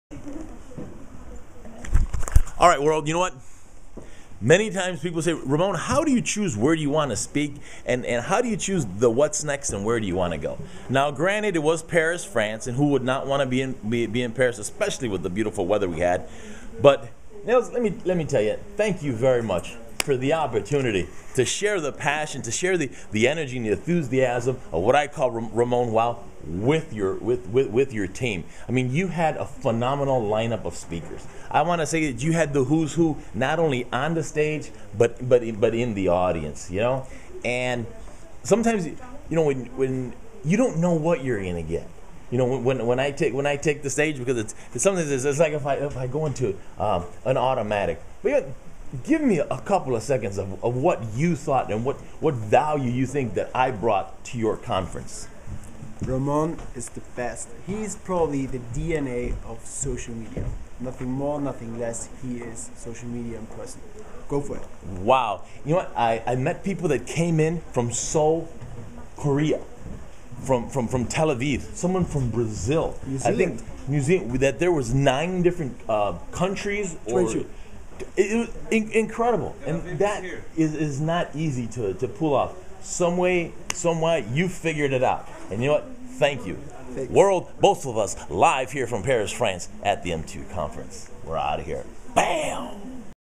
RamonWOW chats with Nils Andres at the M2C conference in Paris '11
Honored to have been dubed "The Unofficial Keynote"